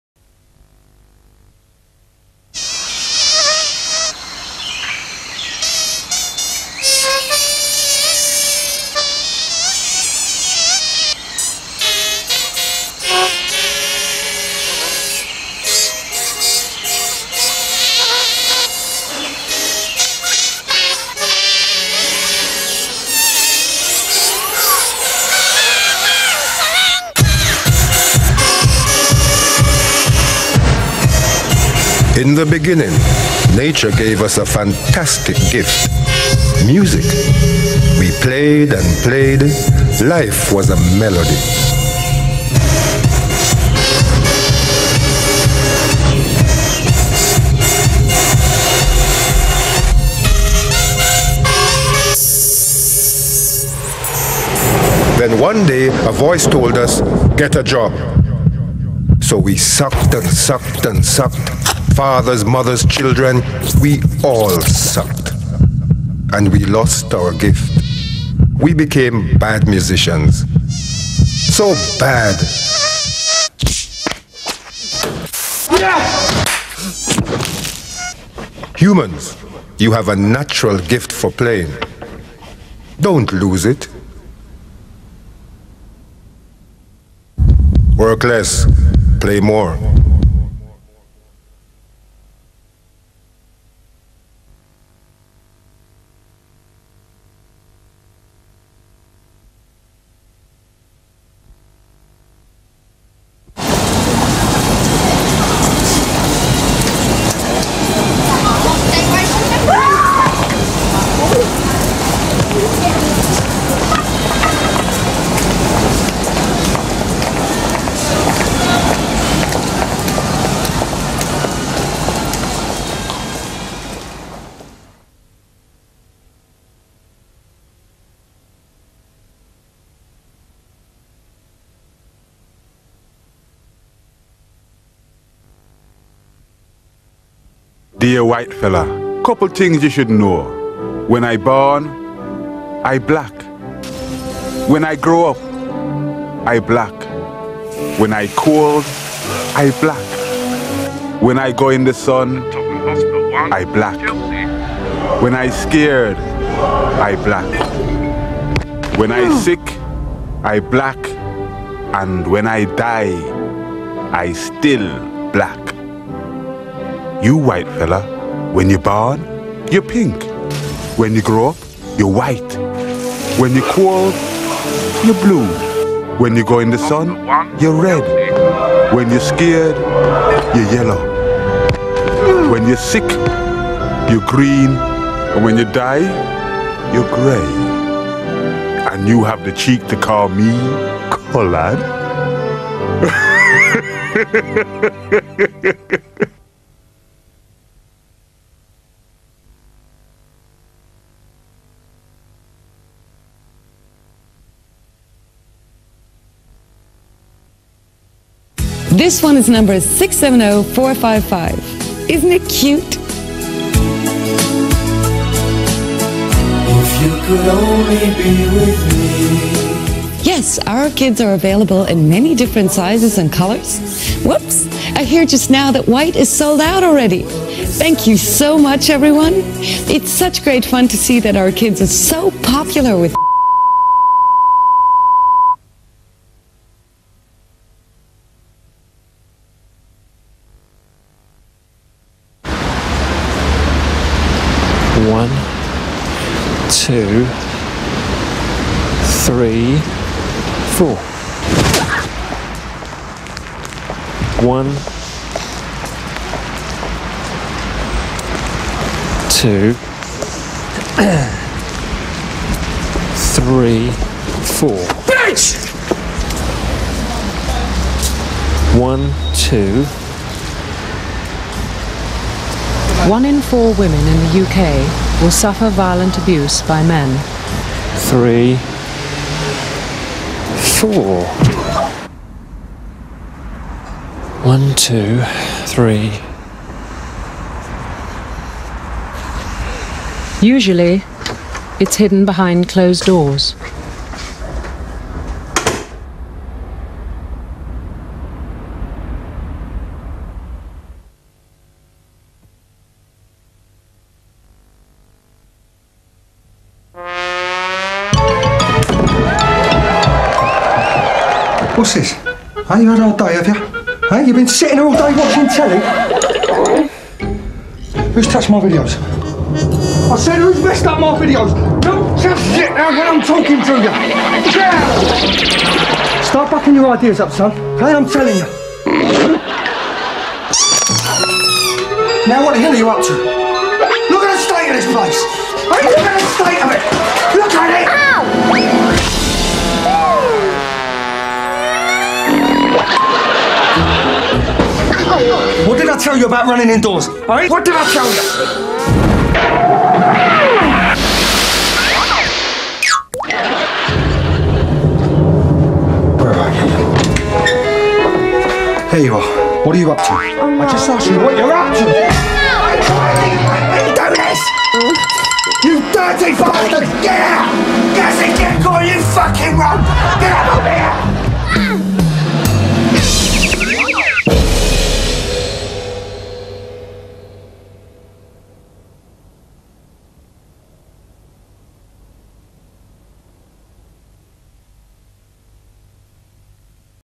Lions Cannes Reklam Yarışması-5
2002 tarihli yaratıcılık yarışmasına katılan reklamlardan bazılarını paylaşıyorum.